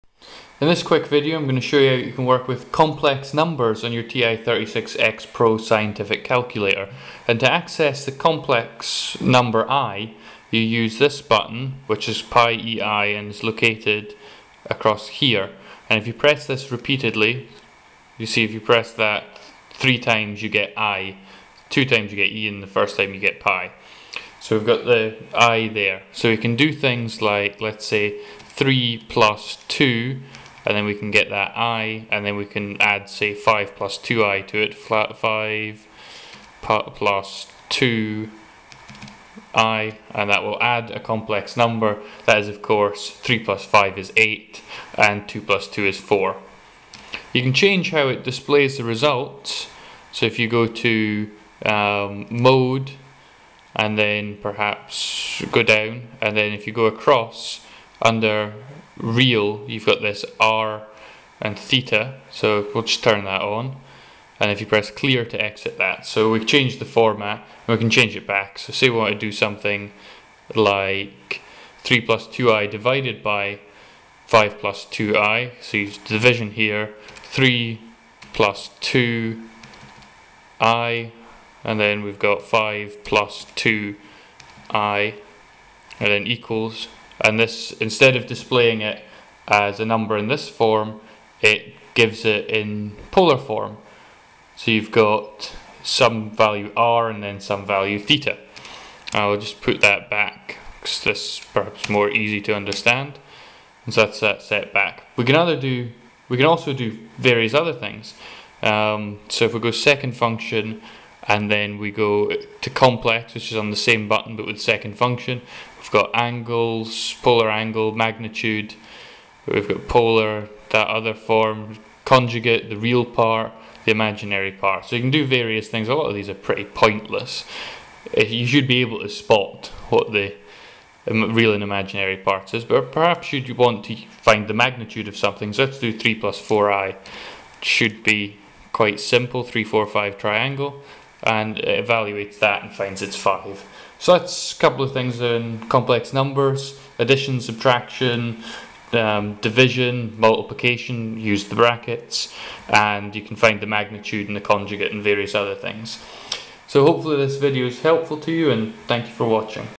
TI-36x Pro Complex Numbers
This video will show you how you can input and work with complex numbers on the TI-36X Pro. We will look at addition,  division and the magnitude to complex numbers.